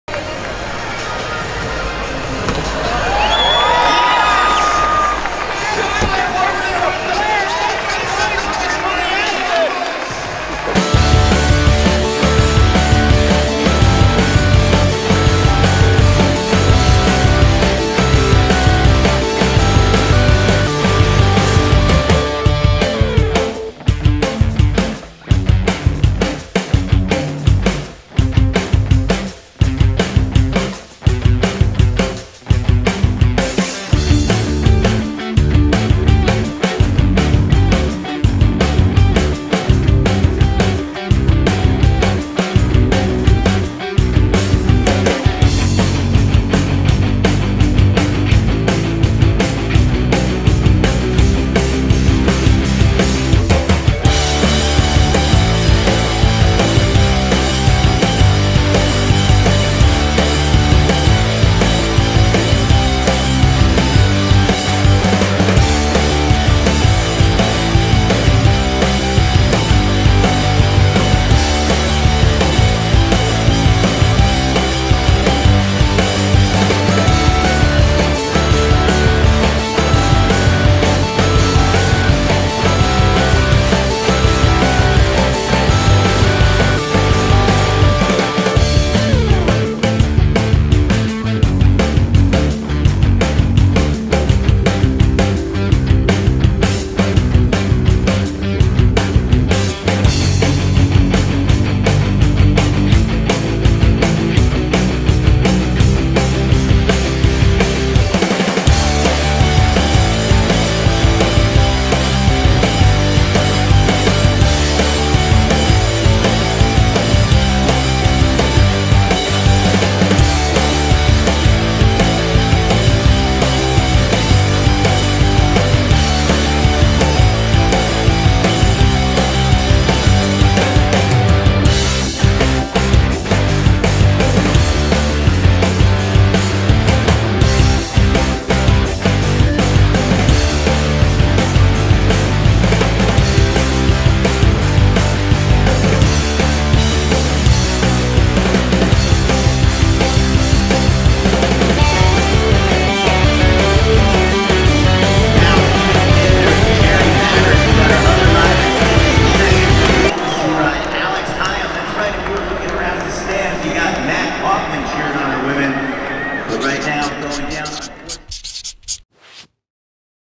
X Games 2018: Women's Jam
Watch the ladies shred the 2018 X Games course. Riders include Angie Marino, Nina Buitrago, Maca Perez, Elizaveta Posadskikh, Charlotte Worthington, and more!